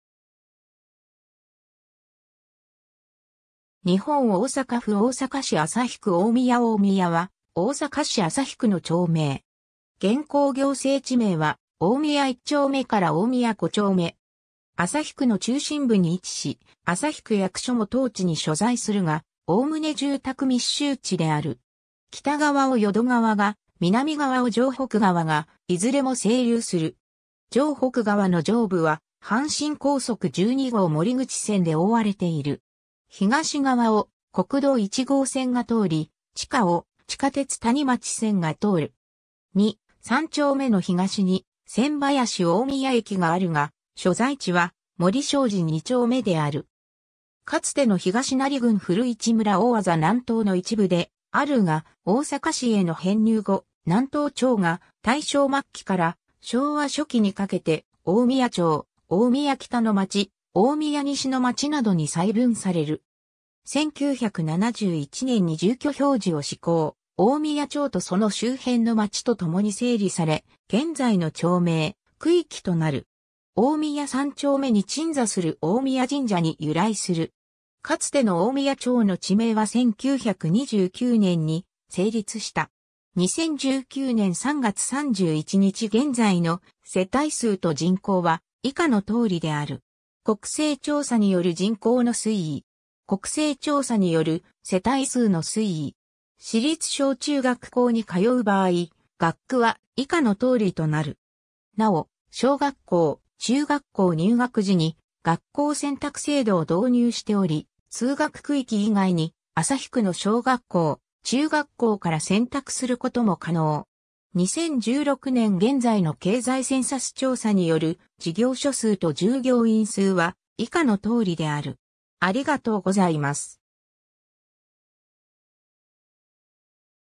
大宮 (大阪市旭区)
大阪市旭区の町・字
日本 」 大阪府 」 大阪市 」 旭区 」 大宮 大宮（おおみや）は、大阪市旭区の町名。
現行行政地名は大宮一丁目から大宮五丁目。
 旭区の中心部に位置し、旭区役所も当地に所在するが、おおむね住宅密集地である。
（地図 - Google マップ） 北側を淀川が、南側を城北川がいずれも西流する。
城北川の上部は阪神高速12号守口線で覆われている。
 東側を国道1号線が通り、地下を地下鉄谷町線が通る。
2,3丁目の東に千林大宮駅があるが、所在地は森小路2丁目である。
 かつての東成郡古市村大字南島の一部であるが、大阪市への編入後、南島町が大正末期から昭和初期にかけて大宮町、大宮北之町、大宮西之町などに細分される。
 1971年（昭和46年）に住居表示を施行、大宮町とその周辺の町とともに整理され、現在の町名・区域となる。
 大宮3丁目に鎮座する大宮神社に由来する。
 かつての大宮町の地名は1929年（昭和4年）に成立した。
 2019年（平成31年）3月31日現在の世帯数と人口は以下の通りである。
なお、小学校・中学校入学時に学校選択制度を導入しており、通学区域以外に旭区の小学校・中学校から選択することも可能。
 2016年（平成28年）現在の経済センサス調査による事業所数と従業員数は以下の通りである。